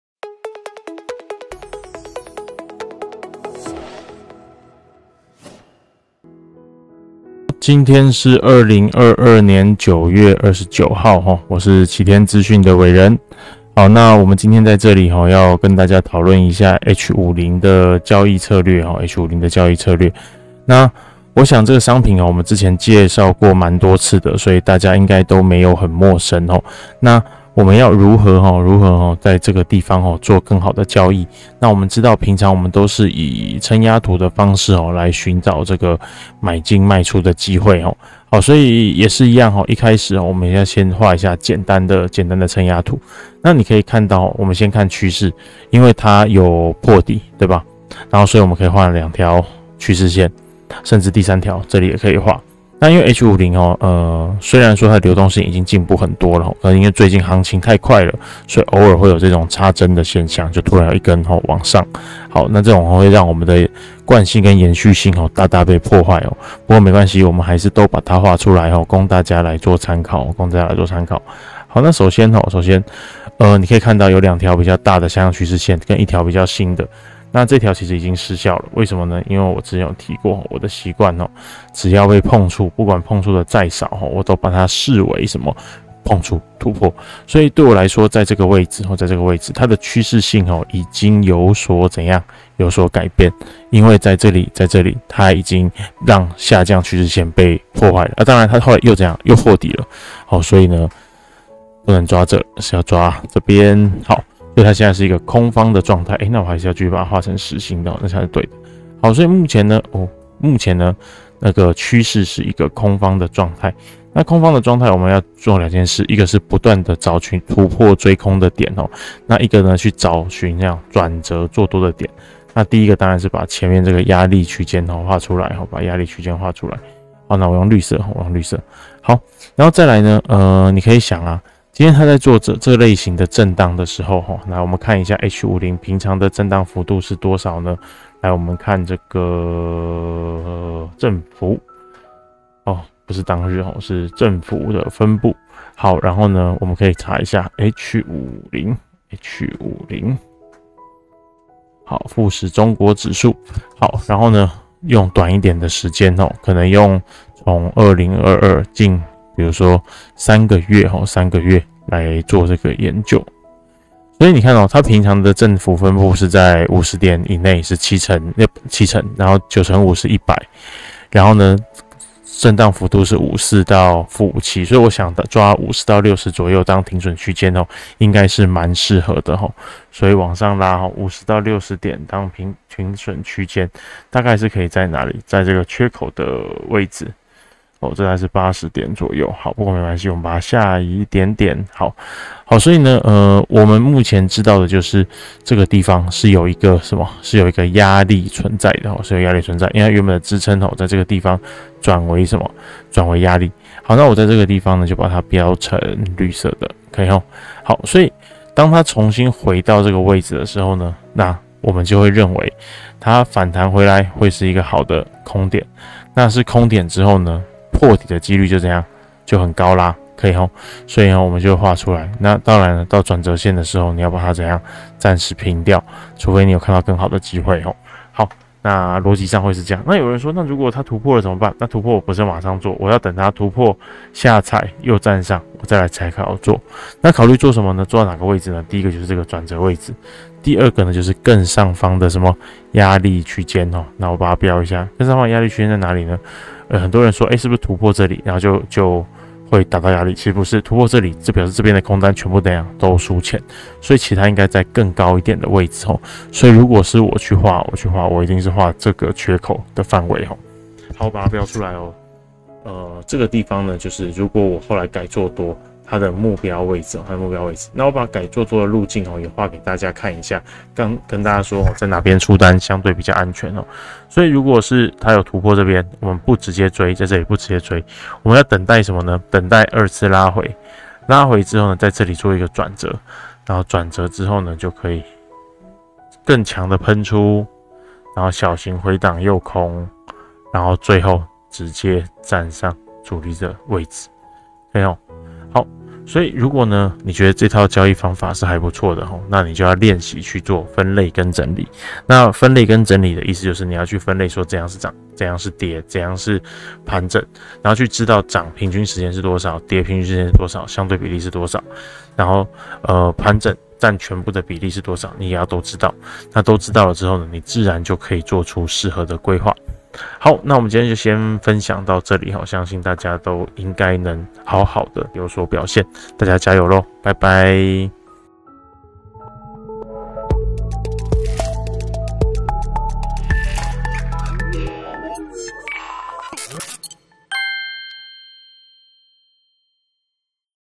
【盤前規劃】 H50－9月份盤前規劃
每月一次的H50盤前規劃又來啦！
這次就讓我們一樣從震盪幅度著手，
跟著Ｏ大逐步畫出完整的撐壓規劃吧～
🔥 還沒試用過期天系統？
現在就領取７天免費試用，體驗即時籌碼的魅力～